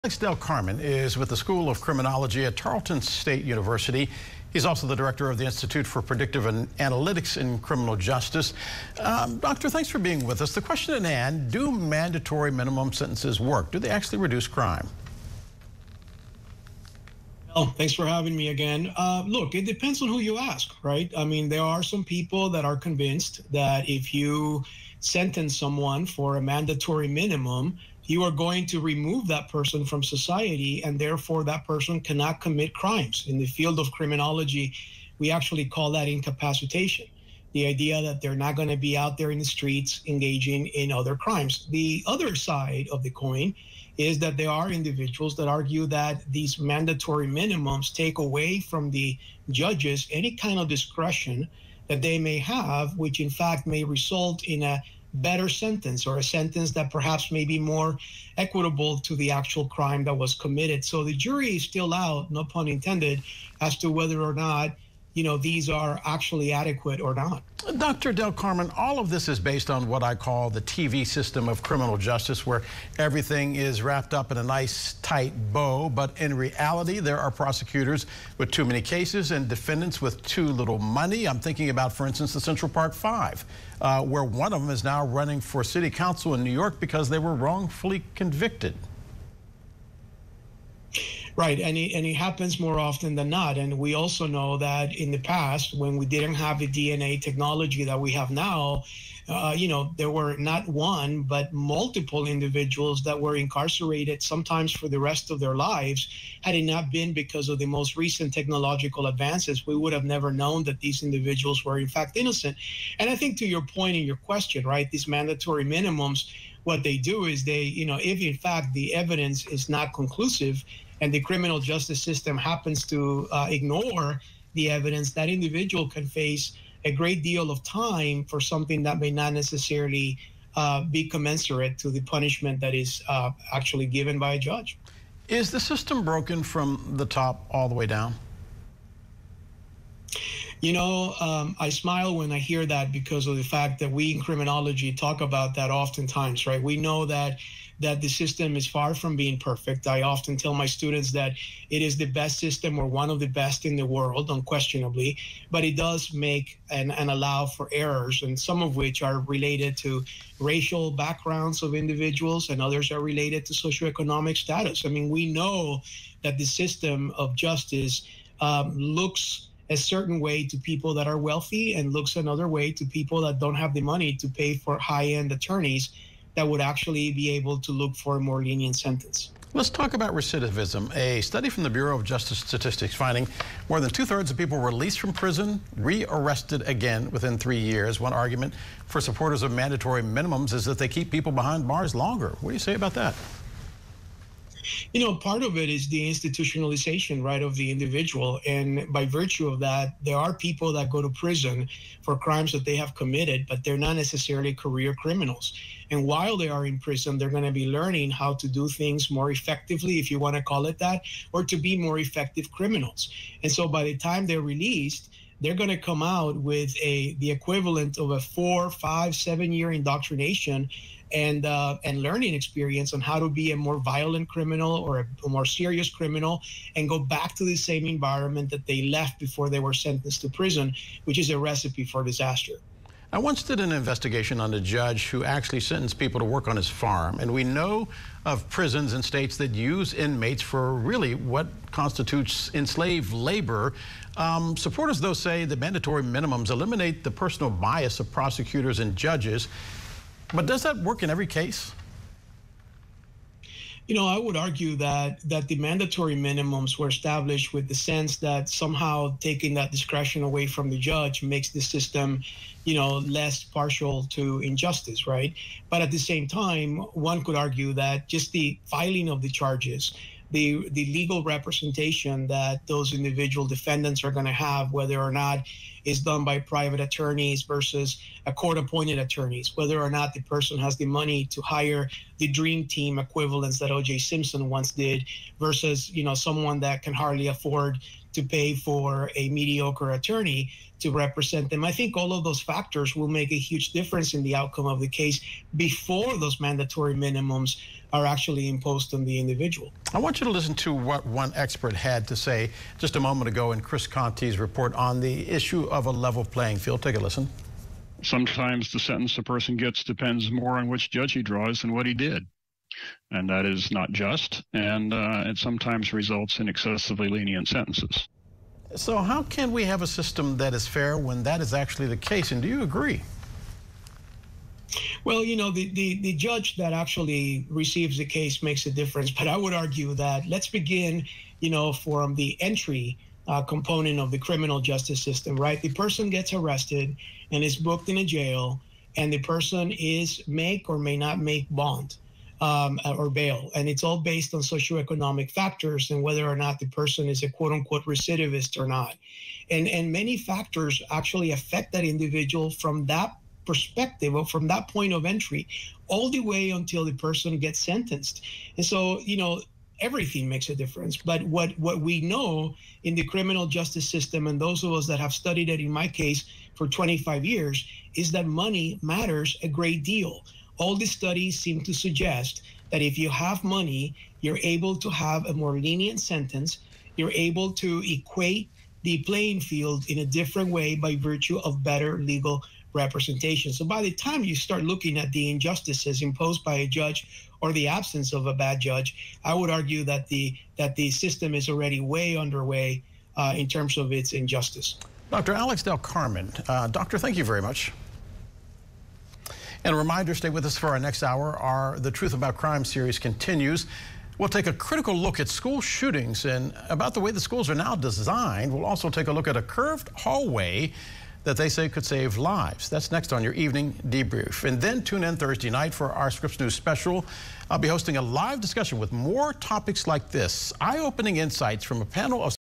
Renewed national debate over mandatory minimum prison sentencing requirements
Scripps News asked Dr. Alex del Carmen to come on for an interview on 5/16/23.

Dr. Alex del Carmen, Associate Dean and Professor at Tarleton State University in Fort Worth, Texas.